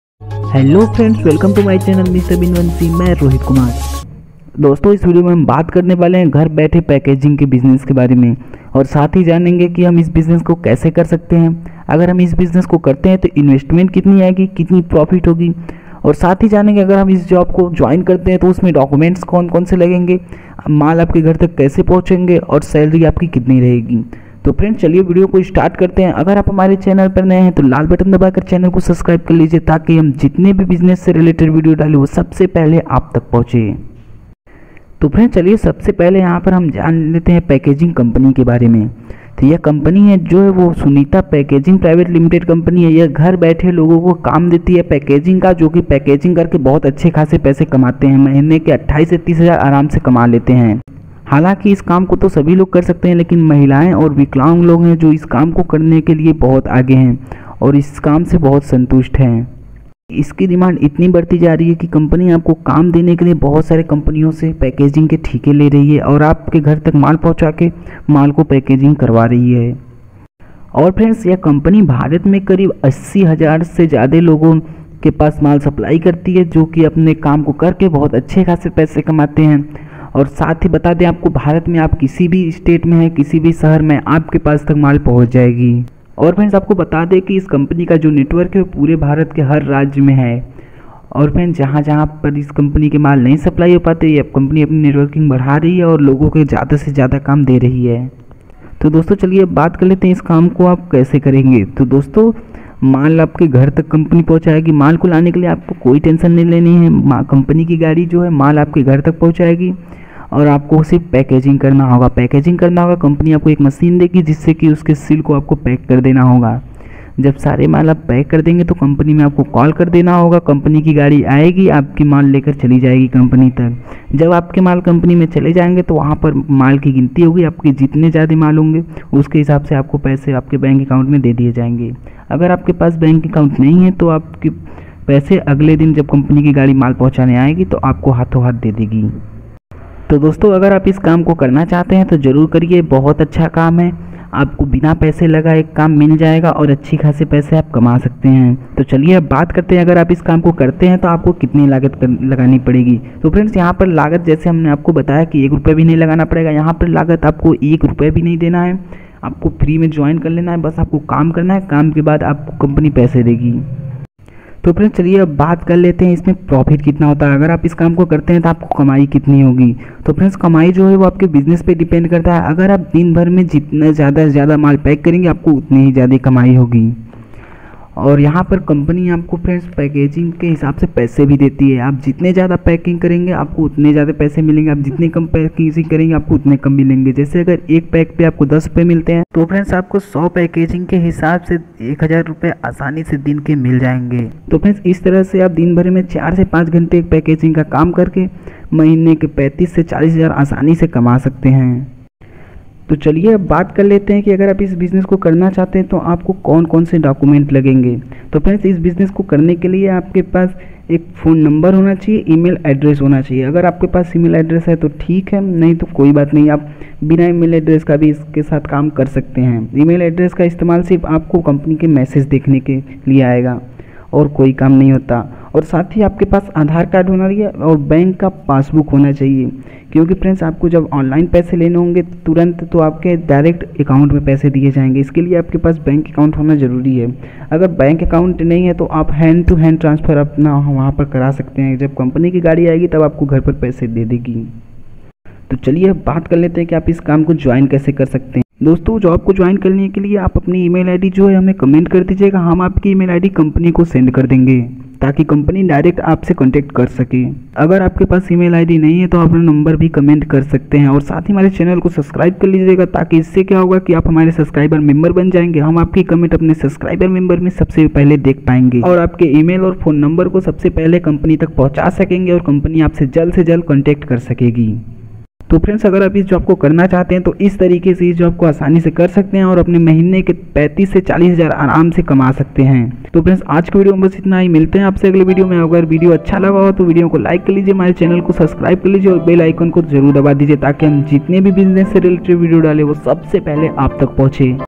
घर बैठे पैकेजिंग बिजनेस करके ₹35000 महीना कमाए।।Packaging business work from home।।घर बैठे पैसे कमाए
दोस्तों इस वीडियो में हम आपको बताएंगे कि आप घर बैठे पैसे कैसे कमा सकते हैं अपना खुद का बिजनेस स्टार्ट कर सकते हैं वह भी बिना पैसे लगाए।

अगर आप इस बिजनेस को करना चाहते हैं तो वीडियो को पूरा देखें वीडियो में हम बताएंगे कि आप कंपनी से कांटेक्ट कैसे कर सकते हैं।

कंपनी से जुड़ने के लिए वीडियो को पूरा देखें वीडियो में पूरी जानकारी है।

पैकेजिंग का बिजनेस कैसे करें